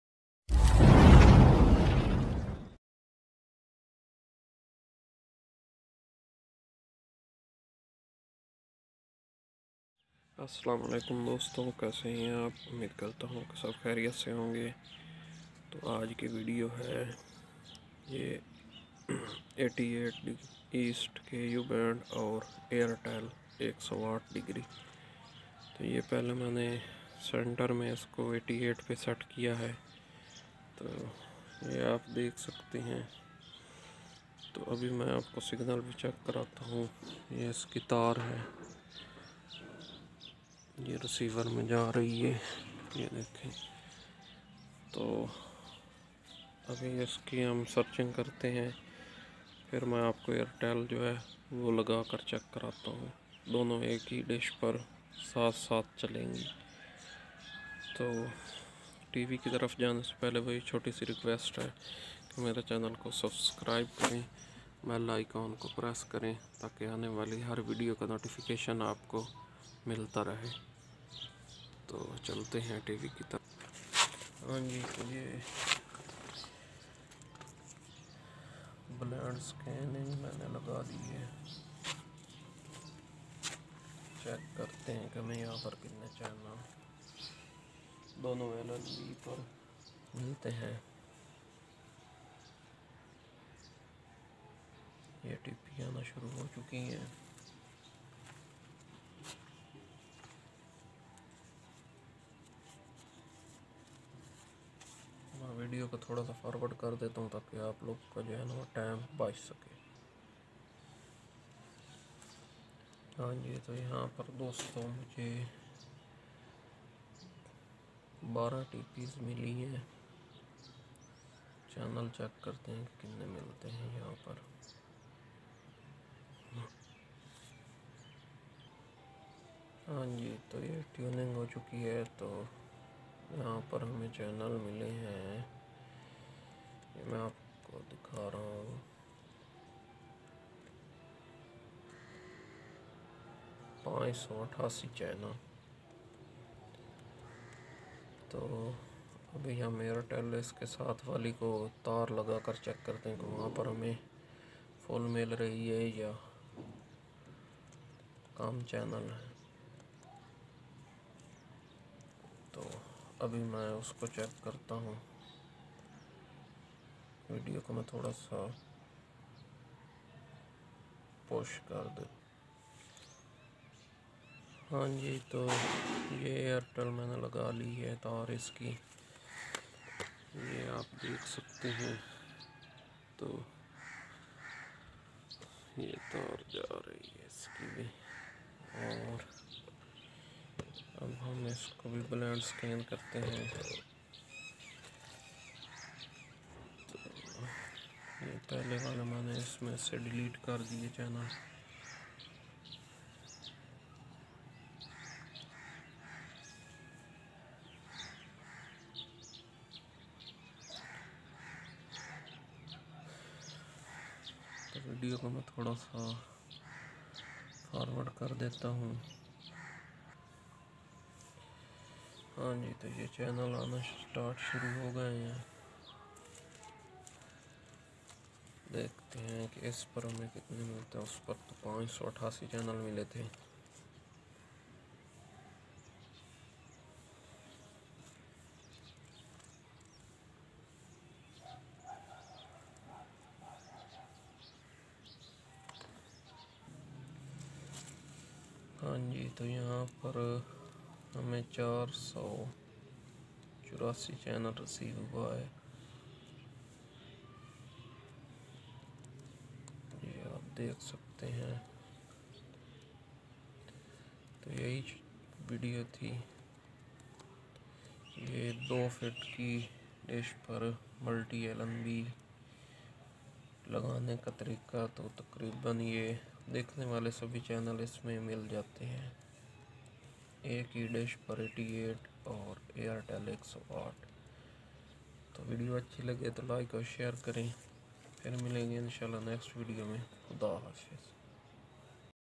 Multti LNB 2Fit Dish setting
Airtel 108 East all channel receive side Lnb and Santar ST 2 88 EAST FULL RECEIVE BEST DISH SETTING 2 FEET DISH IN PAKISTAN SARGODHA